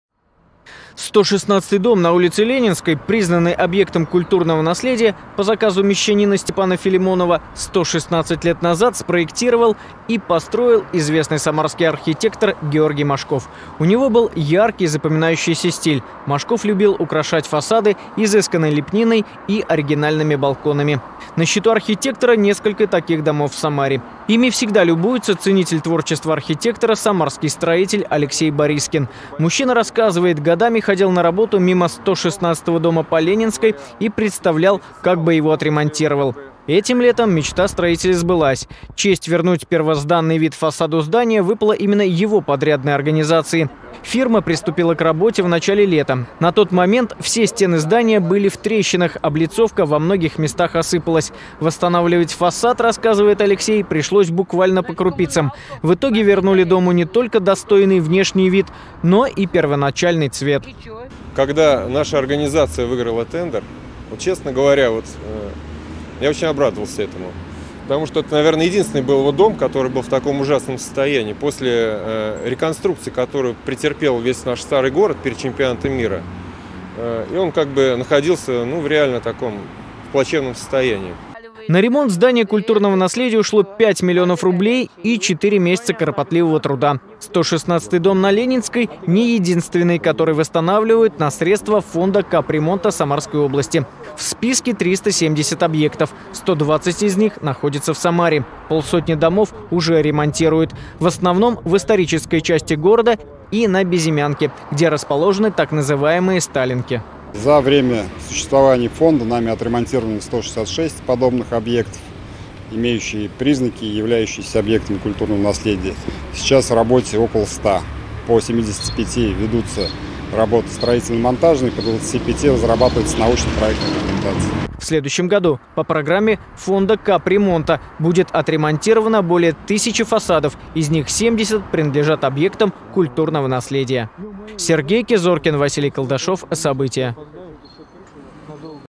В Самаре реставрируют объекты культурного наследия
Самарские фасады преображаются. В городе капитально ремонтируют объекты культурного наследия. Наши корреспонденты узнали, как изменился облик дома мещанина Степана Филимонова по адресу Ленинская, 116. Об этом наш следующий сюжет.